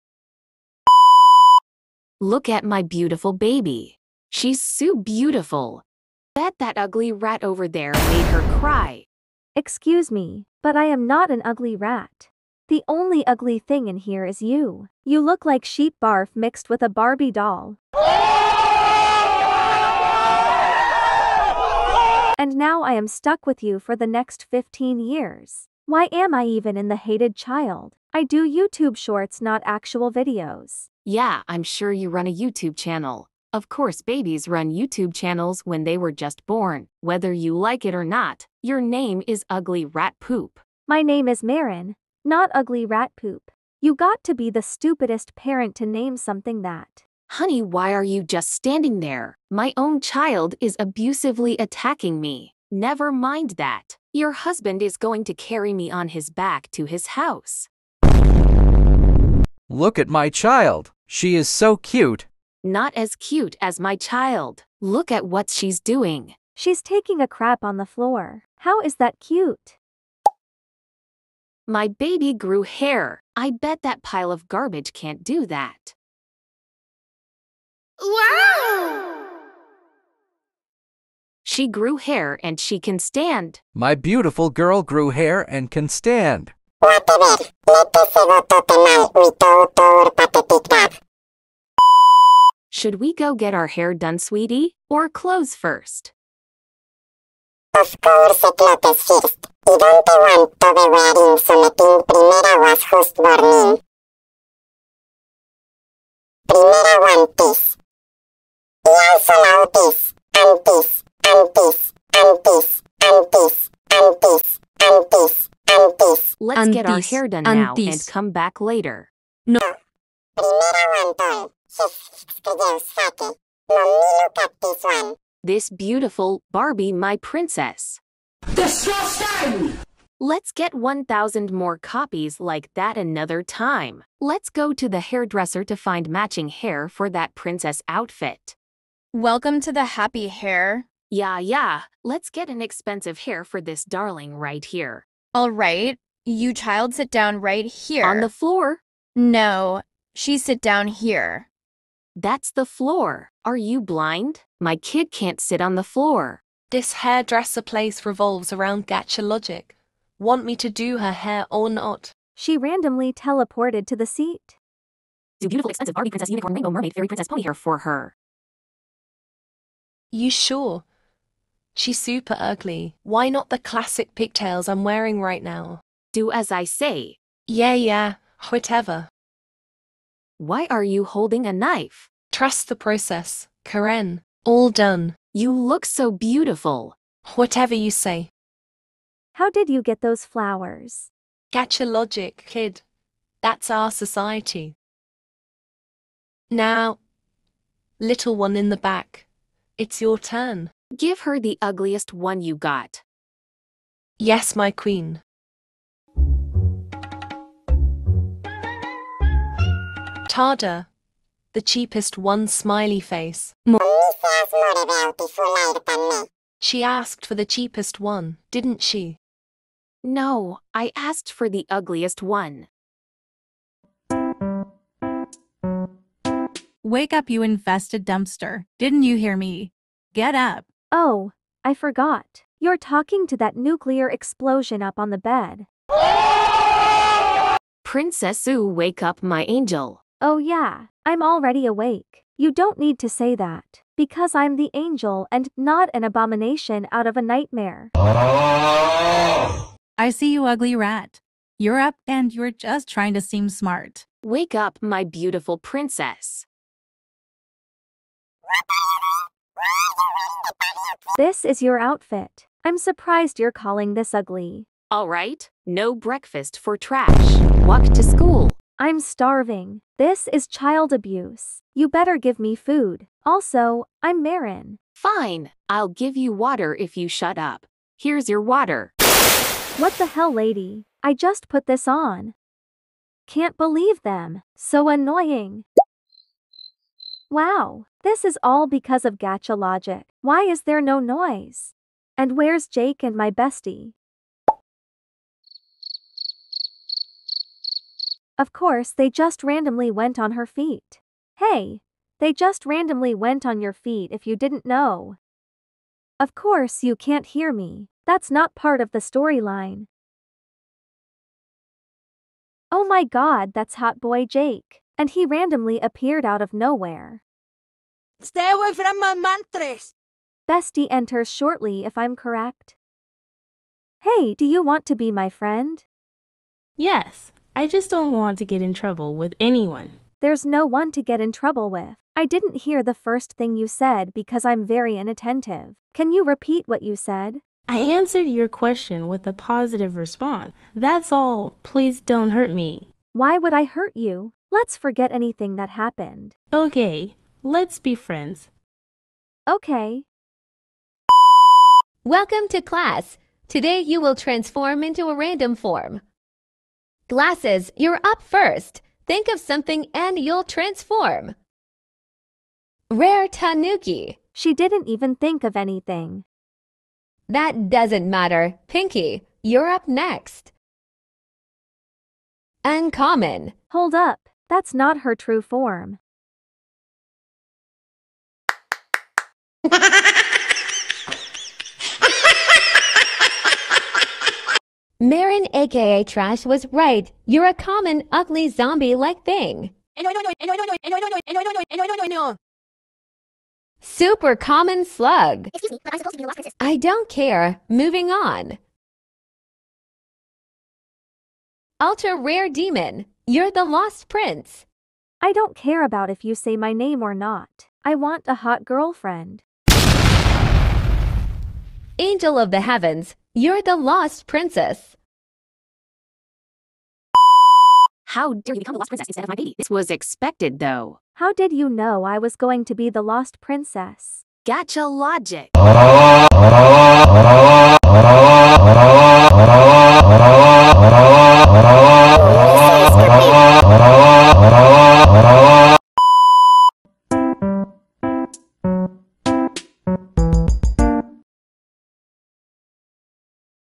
If my oc was in the hated child
This is based of the trend in 2018 or something. It died out in like 2021 I think.

Let’s revive it! :)

Music creds: ​⁠@TommyInnit

Gacha hated child, Crazy gacha video, ​⁠@marinrin.e Funny gacha video, Gacha, Funny, Comedy, etc

0:00 loading screen

0:01 the story begins

1:33 mall

2:19 cringey hair saloon

3:45 years later

4:25 bratty mom not giving her kid lunch

4:47 instant avatar change

5:11 Jake appears

5:23 the cringey best friend

5:52 “true” forms

6:20 the true climax

6:25 tbh idk